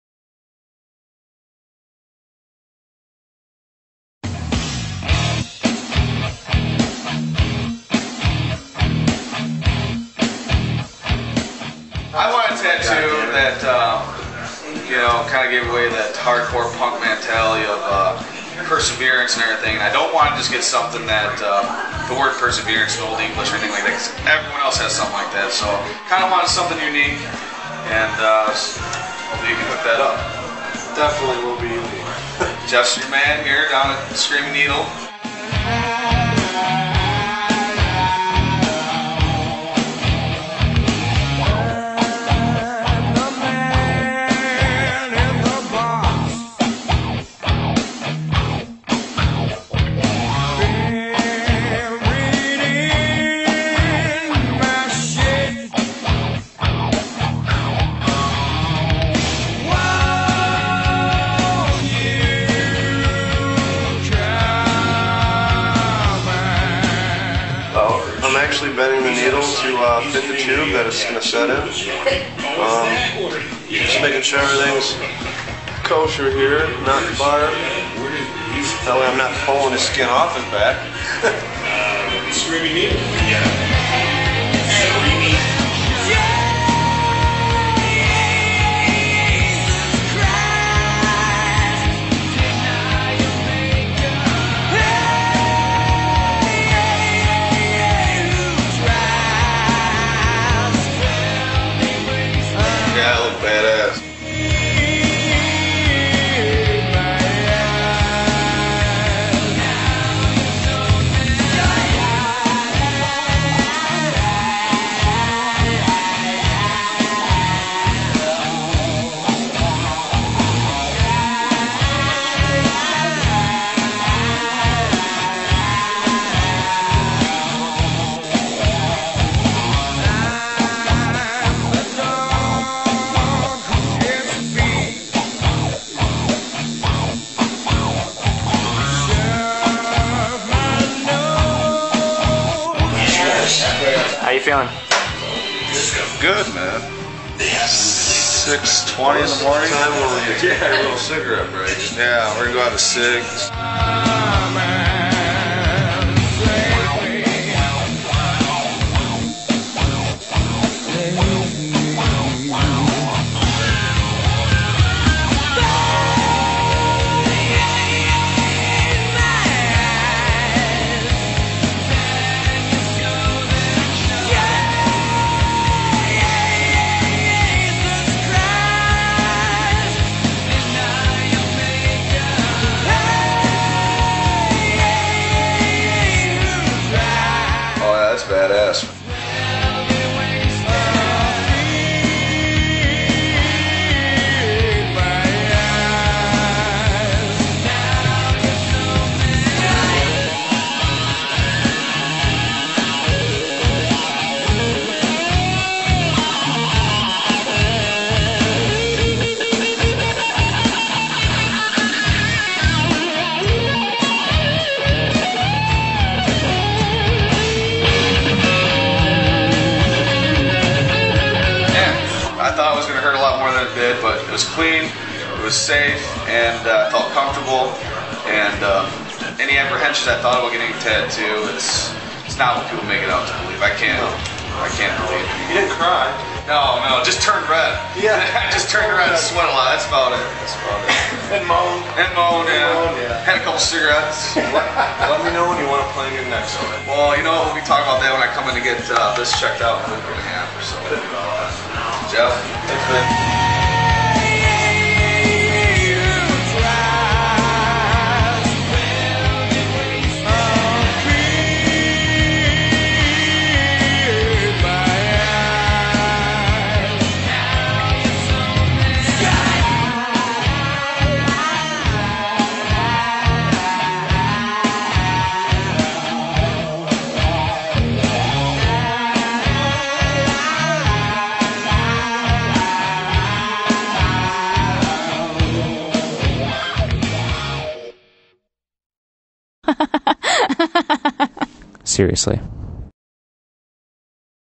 Tattoo 101
A quick and handy guide through getting your first tattoo.